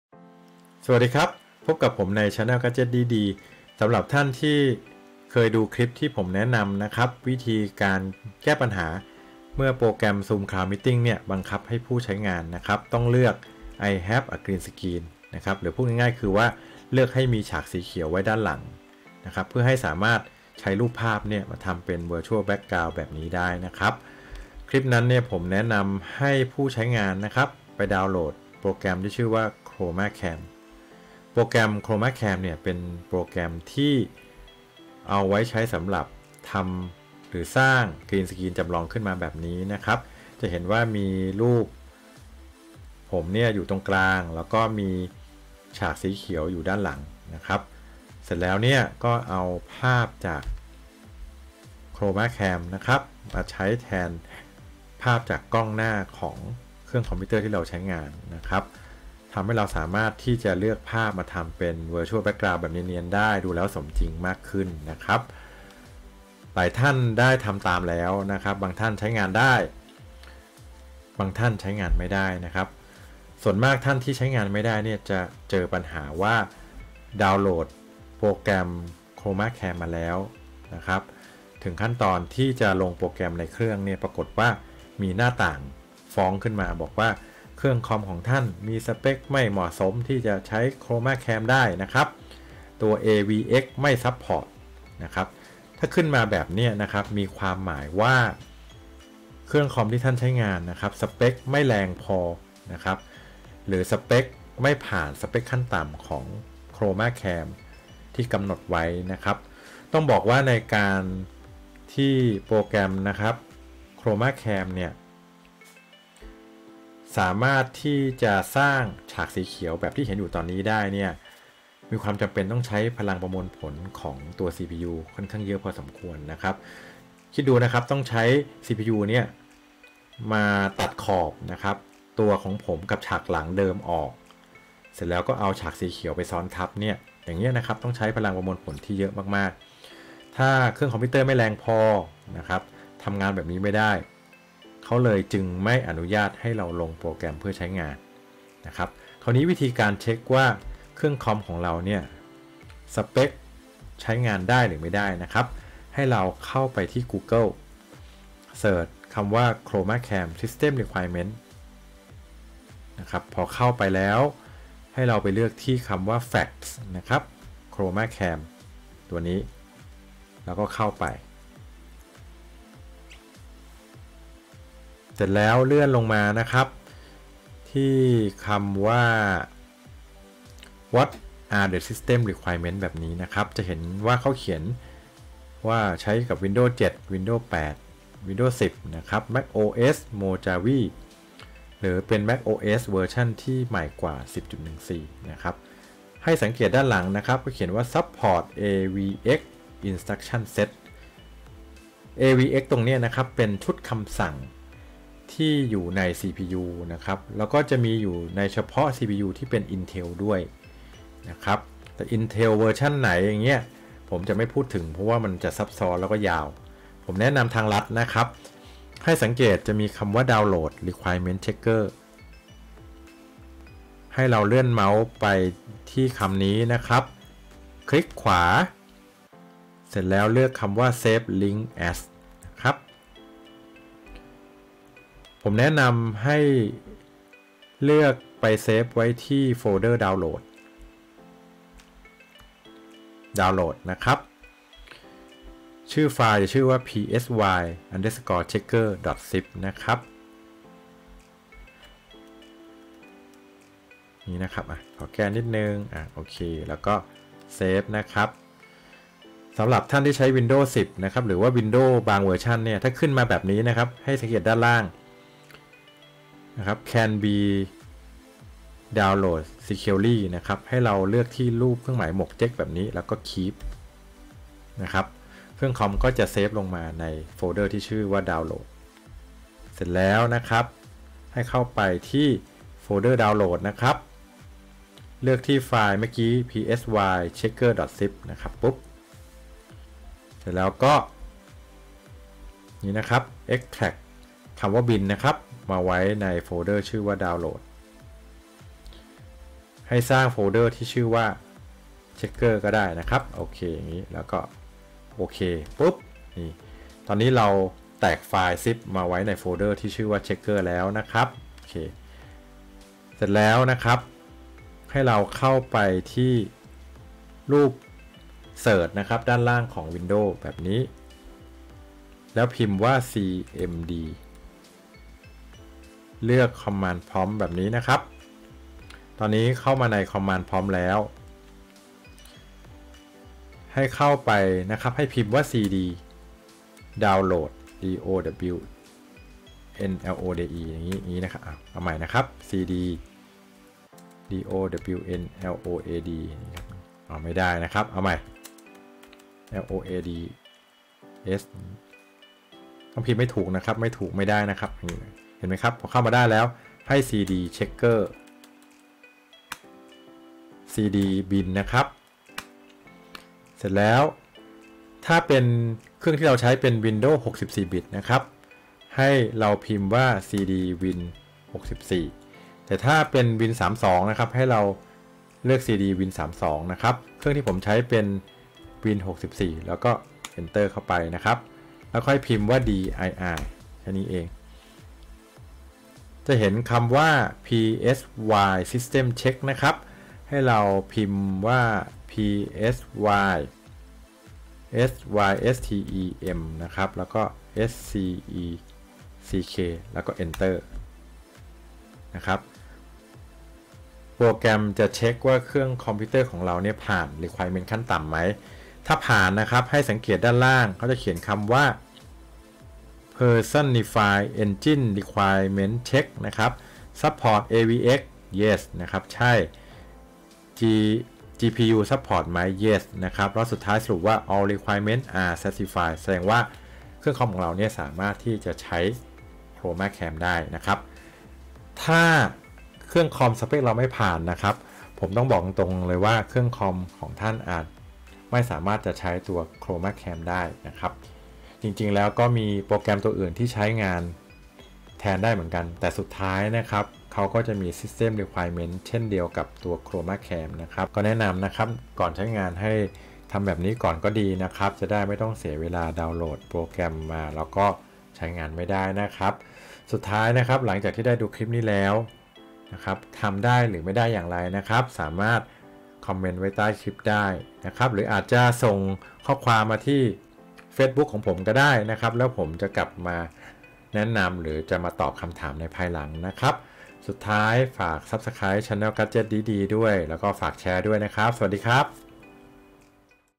Zoom Cloud Meeting |ก่อนใช้ ChromaCam มาเชค Spec. เครื่องกันก่อนด้วยวิธีนี้
คลิป link ด้านล่างหากต้องการซื้อ Green Screen

สำหรับผู้ที่ใช้งาน Zoom Cloud Meeting แล้วโดนบังคับให้เลือก I have a green screen  ผมได้แนะนำโปรแกรม ChromaCam เพื่อช่วยทำ Green Screen จำลอง

แต่ทั้งนี้มีบางท่านสามารถใช้งานได้ บางท่านไม่สามารถใช้ได้ ..ทั้งนี้เนื่องจาก ChromaCam กำหนด spec. ขั้นต่ำของเครื่องคอมที่สามารถใช้งานได้

ผมได้ทำคลิปนี้ขึันมาสำหรับผู้ที่ใช้ WIndows แล้วอยากตรวจสอบ Spec. เครื่องว่าผ่านหรือไม่ผ่าน

หากดูแล้วทำไม่ได้ comment ไว้ใต้คลิปได้เลยครับ